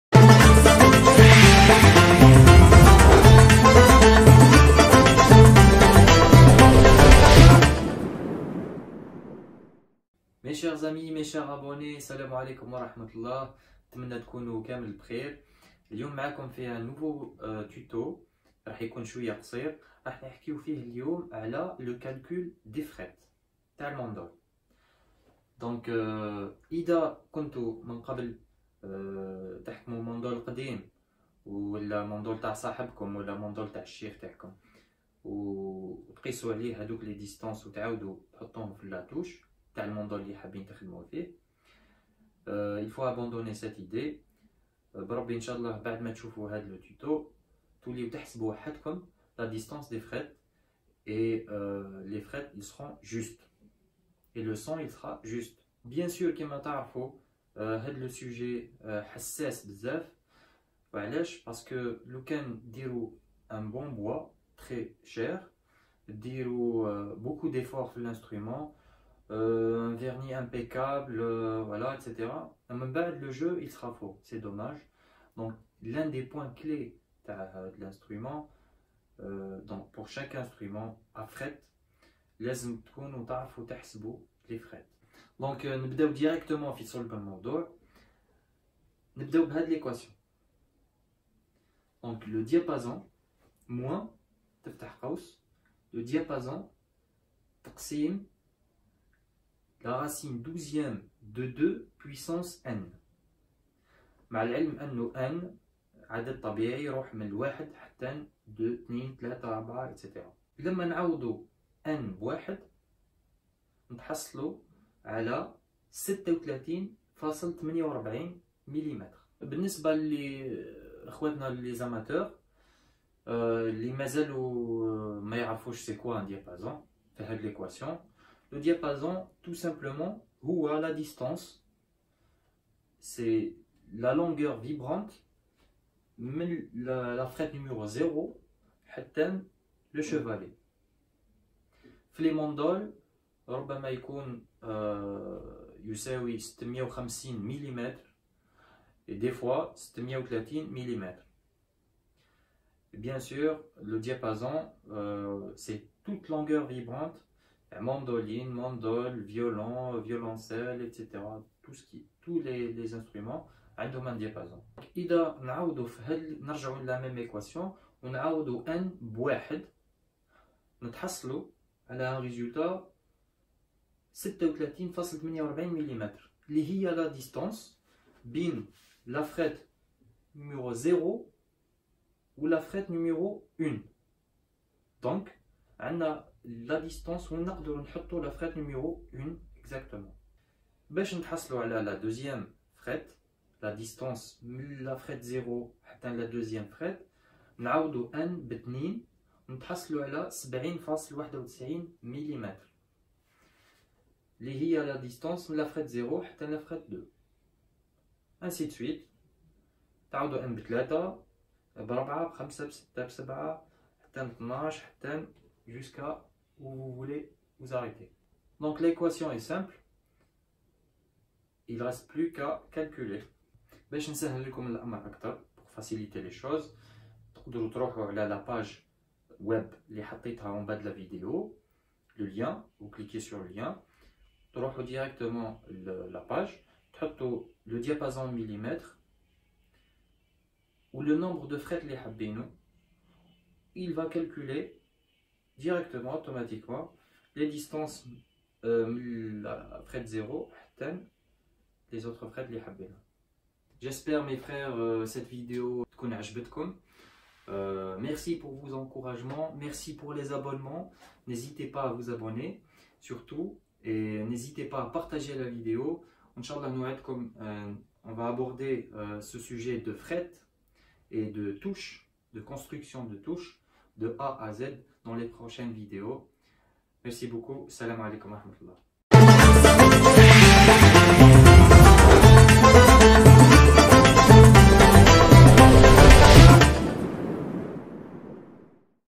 Mandole Algérien, Calcul frettes # 4
Suite à votre demande, j'ai le plaisir de partager avec vous cette vidéo pour savoir comment calculer les frettes.